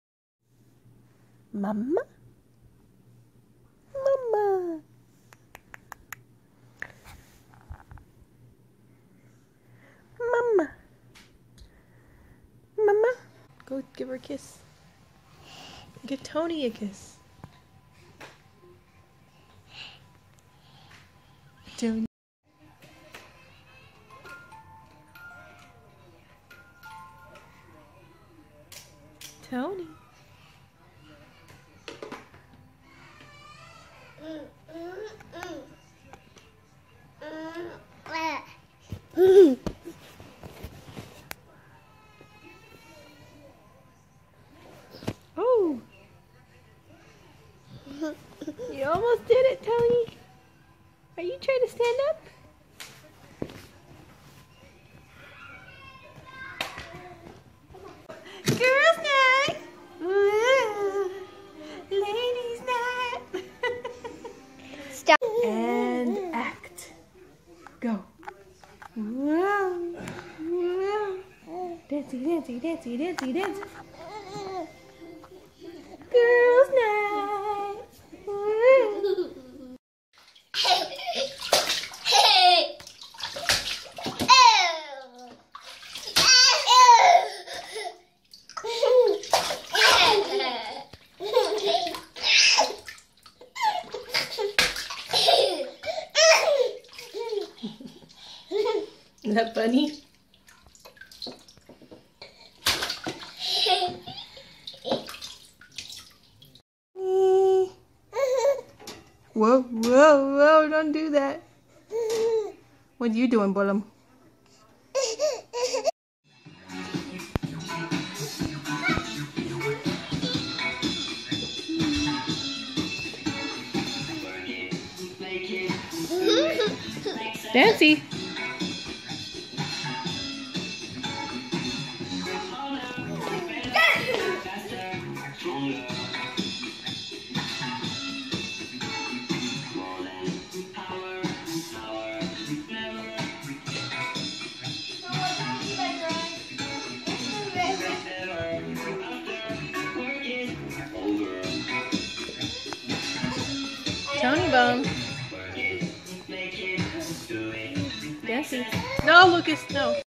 Random Phone Vids--Toni
Toni is standing up at 9 months :)